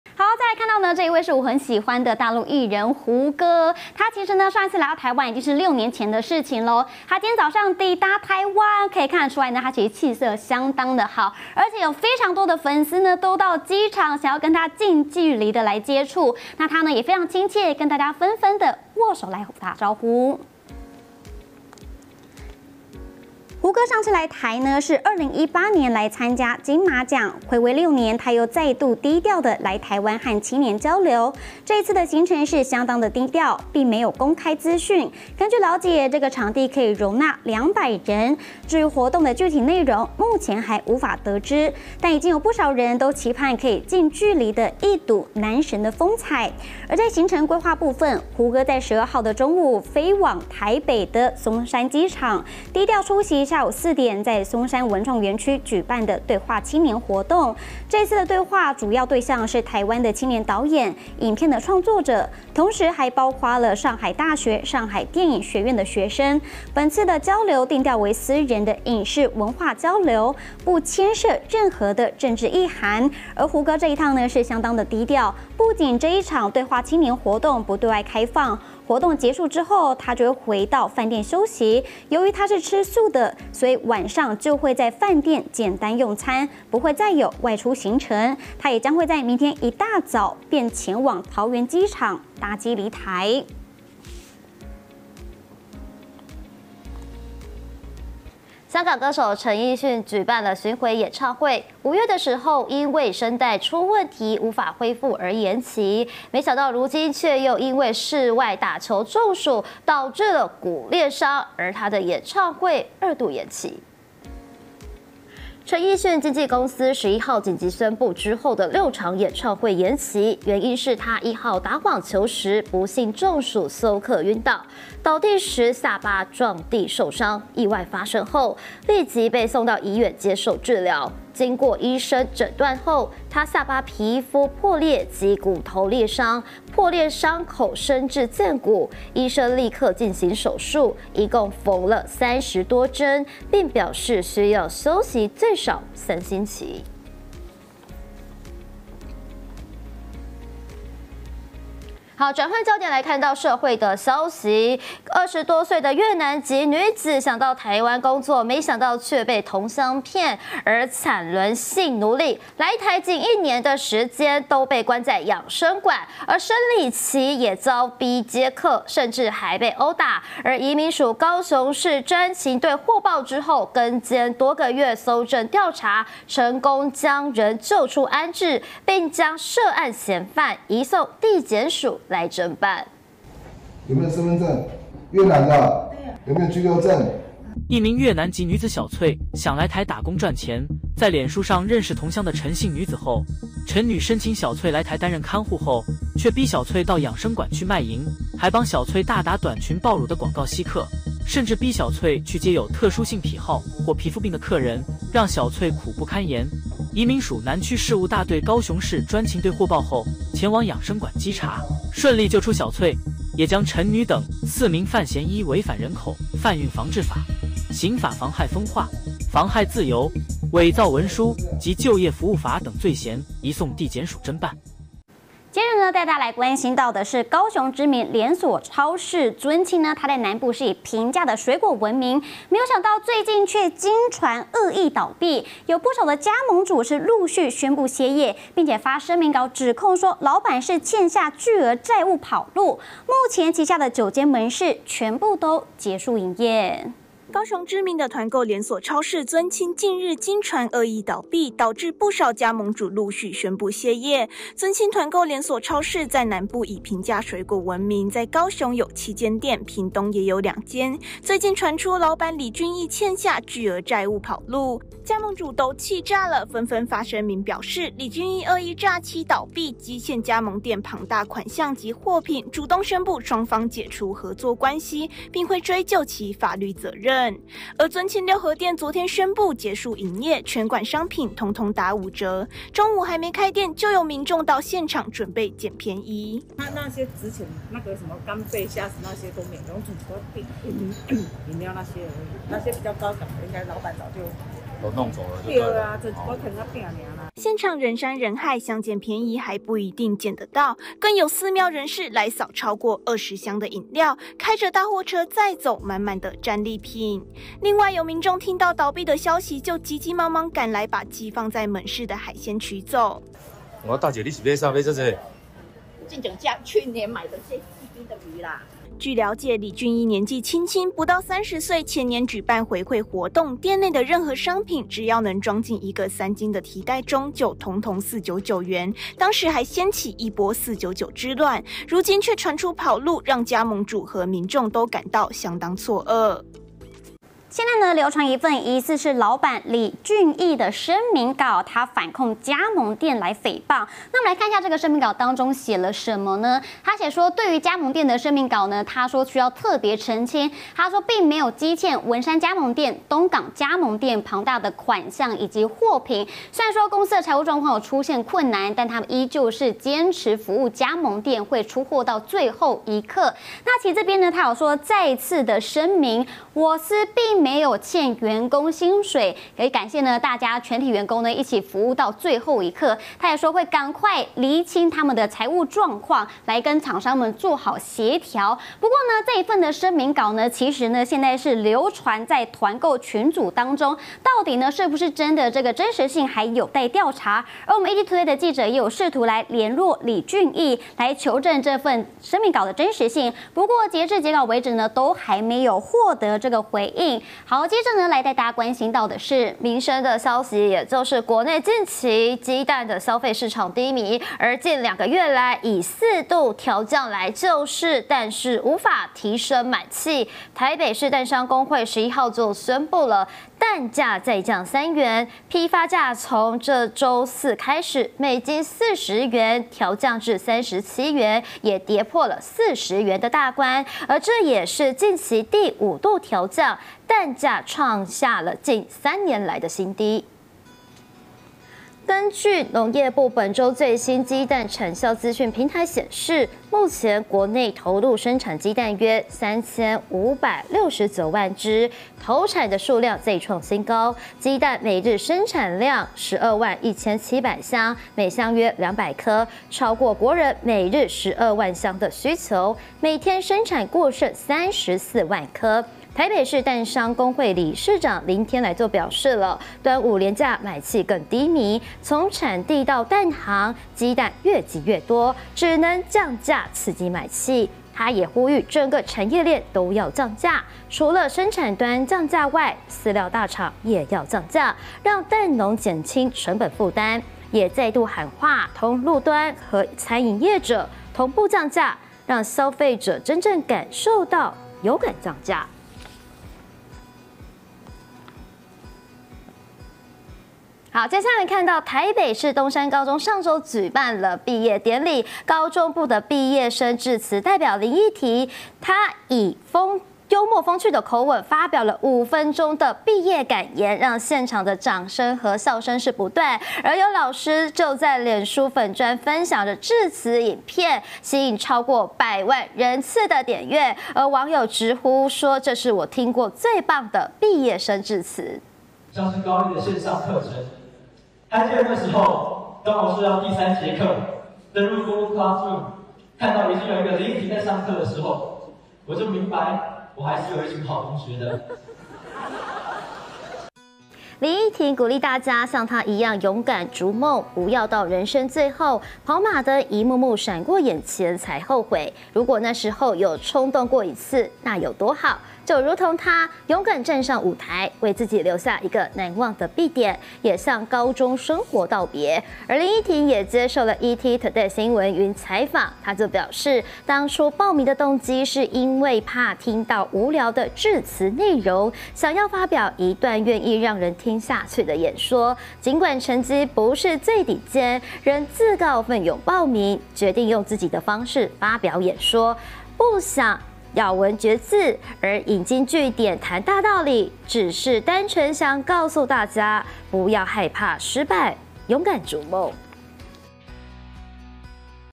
胡歌到台灣了 灰綠西裝超帥現身｜20240612 ET午間新聞
主播 邱筱茜、黃稜涵

新聞內容
胡歌到台灣了 灰綠西裝超帥現身
陳奕迅突休克暈倒 重摔濺血傷可見骨
短裙爆乳廣告吸客 惡雇主逼移工賣淫
獨／老闆跑路 高雄尊青超市9門市全收
尊青超市發聲明 反控加盟店誹謗
蛋價再降3元 公會喊話：餐飲通路也要降
獨／東山高中最強畢業生致詞 破百萬點閱